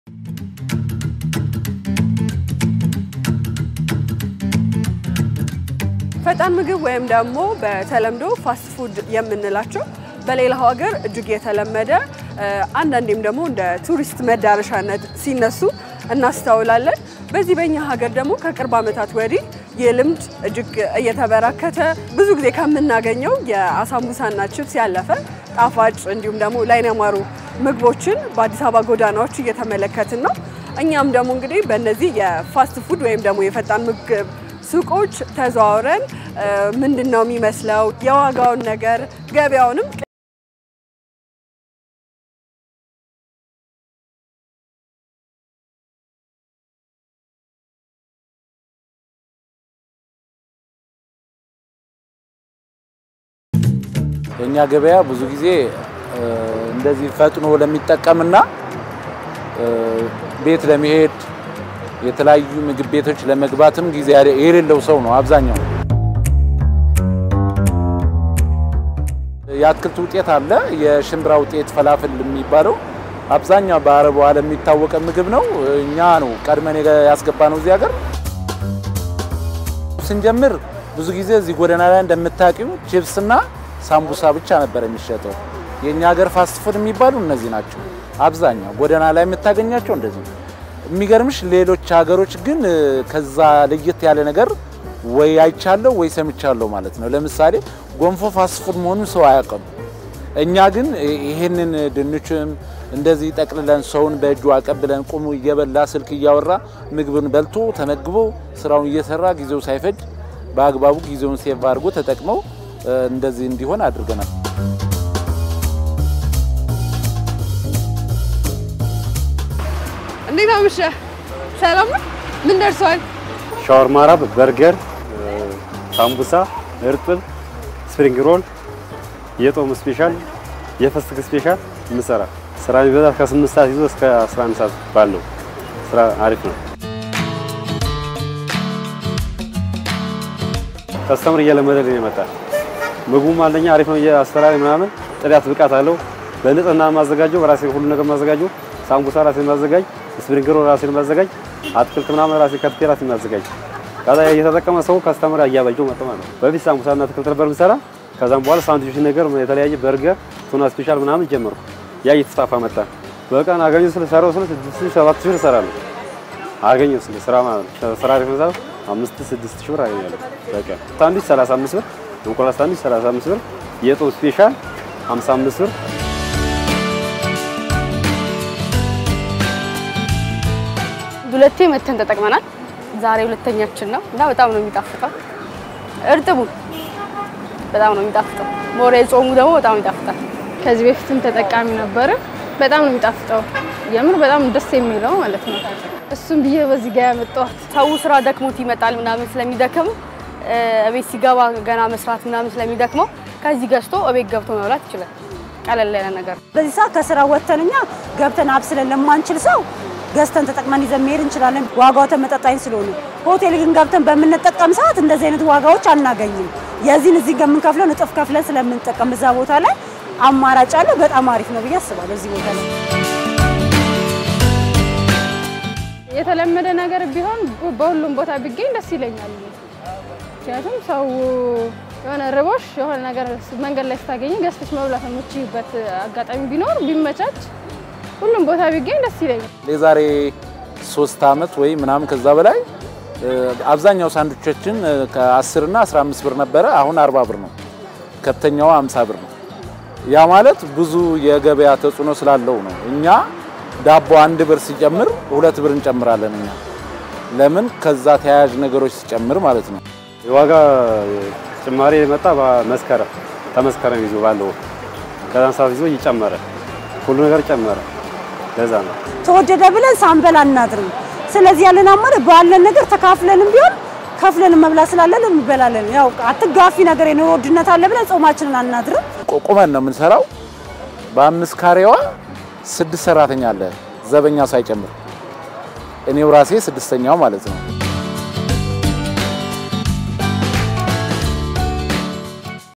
ተፈላጊ እየሆኑ የመጡት ፈጣን ምግቦች
በተለያዩ አካባቢዎች የሚሸጡ ፈጣን ምግቦች አሁን ካለው የኑሮ ውድነት አንጻር በተመጣጣኝ ዋጋ ሚፈልጉትን ገዝተው እየተመገቡ መሆኑን ተጠቃሚዎች ተናግረዋል።